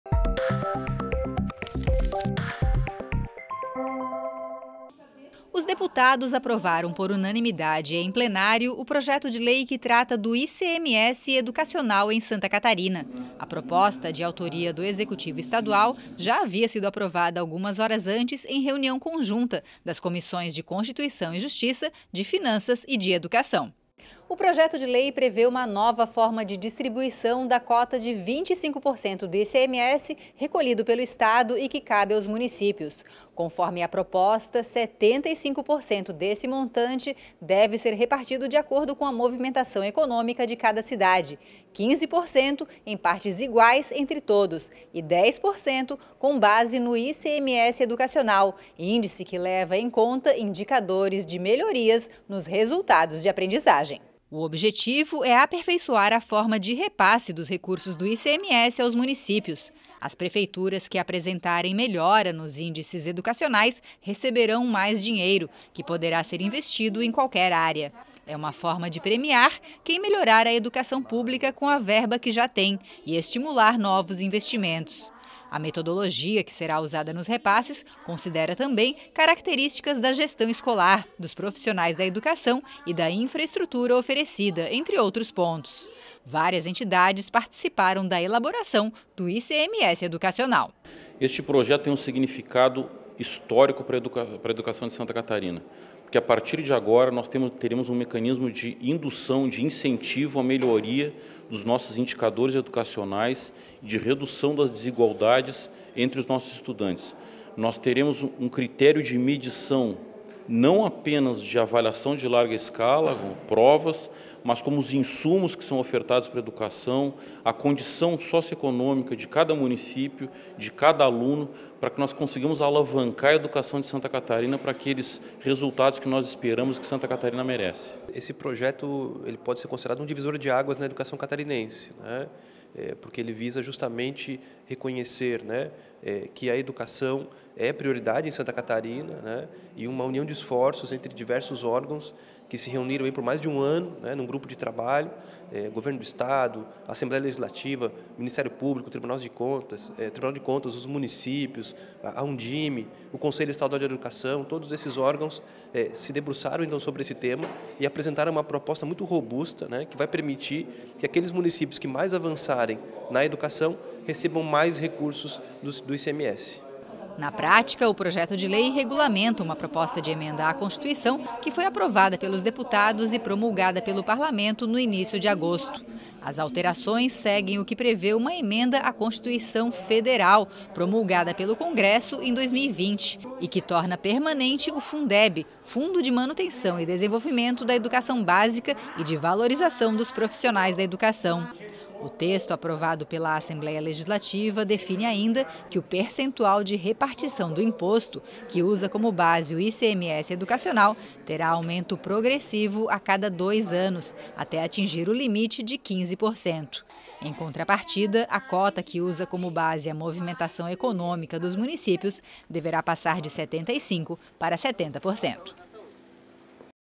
É Notícia - ICMS Educacional é aprovado por unanimidade em Plenário - 16/08/22
Os deputados aprovaram por unanimidade em Plenário o projeto de lei que trata do ICMS Educacional em Santa Catarina. A proposta, de autoria do Executivo estadual, já havia sido aprovada algumas horas antes em reunião conjunta das comissões de Constituição e Justiça, de Finanças  e de Educação.